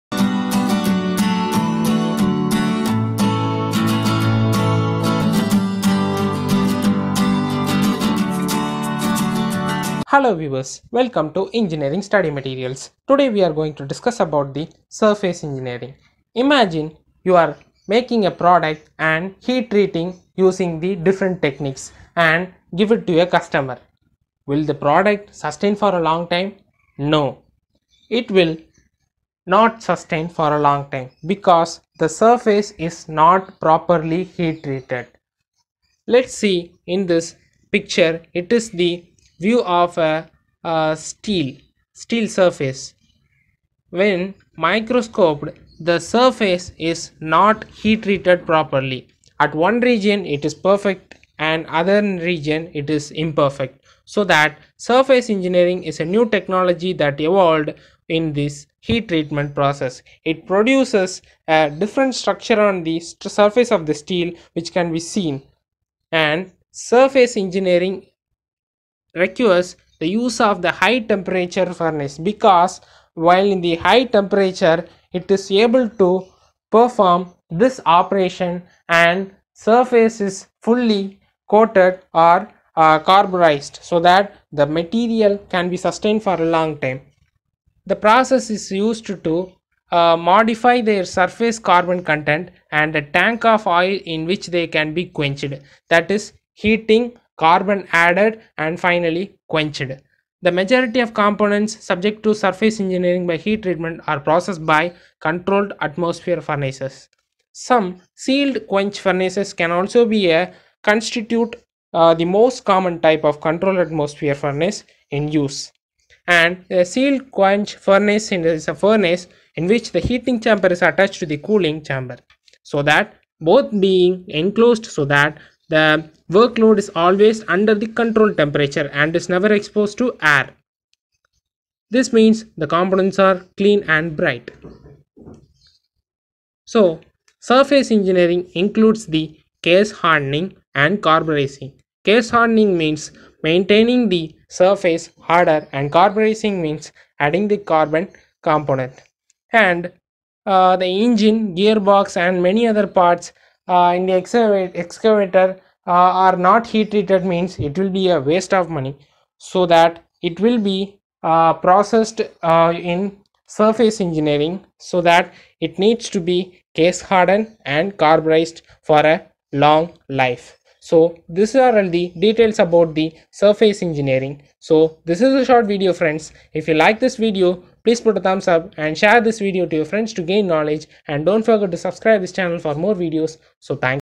Surface Engineering | Definition | Methods | ENGINEERING STUDY MATERIALS
Surface engineering is the sub-discipline of materials science which deals with the surface of solid matter. It has applications to chemistry, mechanical engineering, and electrical engineering (particularly in relation to semiconductor manufacturing).

Solids are composed of a bulk material covered by a surface. The surface which bounds the bulk material is called the Surface phase. It acts as an interface to the surrounding environment. The bulk material in a solid is called the Bulk phase.

The surface phase of a solid interacts with the surrounding environment. This interaction can degrade the surface phase over time. Environmental degradation of the surface phase over time can be caused by wear, corrosion, fatigue and creep.

Surface engineering involves altering the properties of the surface phase in order to reduce the degradation over time. This is accomplished by making the surface robust to the environment in which it will be used. It provides a cost-effective material for robust design. A spectrum of topics that represent the diverse nature of the field of surface engineering includes Plating technologies, Nano and emerging technologies and Surface engineering, characterization and testing.